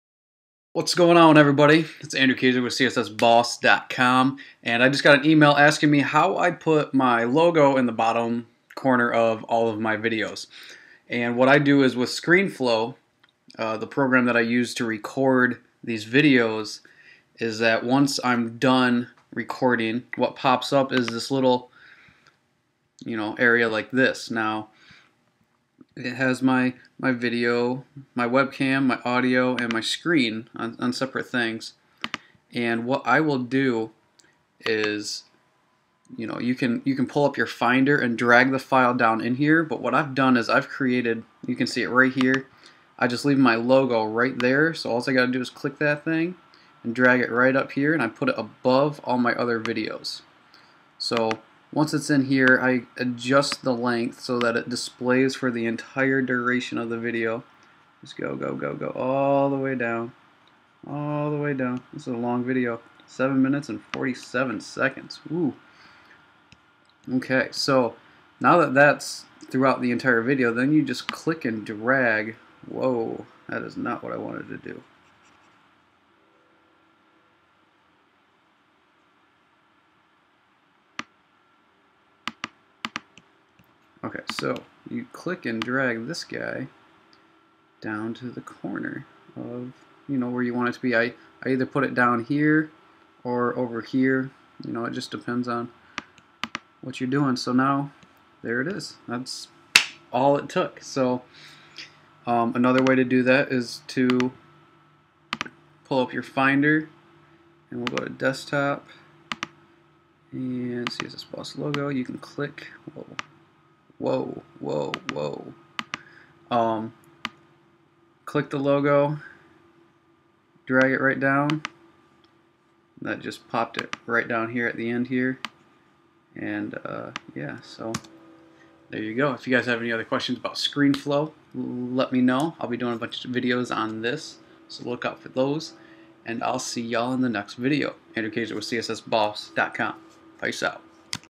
Adding A Water Mark In ScreenFlow
This is how I ad my logo watermark to all of my videos with screenflow

If I helped you at all and you want to show some support,